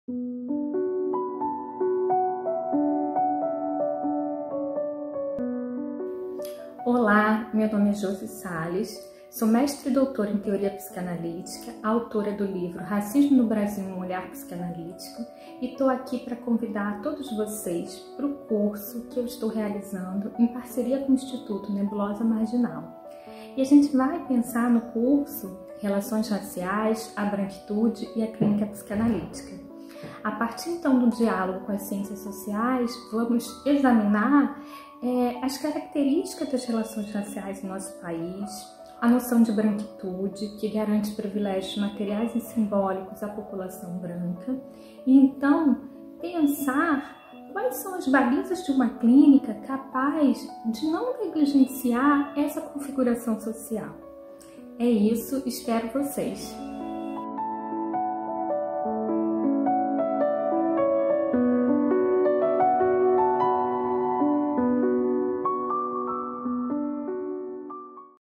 Relações Raciais, Branquitude e Clínica Psicanalítica no Brasil
Instituto Nebulosa Marginal apresenta:

Curso: Relações Raciais, Branquitude e Clínica Psicanalítica no Brasil
Ministrante: Jôse Sales
Duração: 04 horas
Investimento: R$ 88,00
OBS: o conteúdo fica disponível por 8 dias para quem se inscrever depois do início dos encontros ou não puder assistir ao vivo e possui certificado

Parte do que for arrecadado será revertido em doações para a ONG:
Casa Nem, Casa Viva (RJ)

Mais informações: MarginalNebulosa.com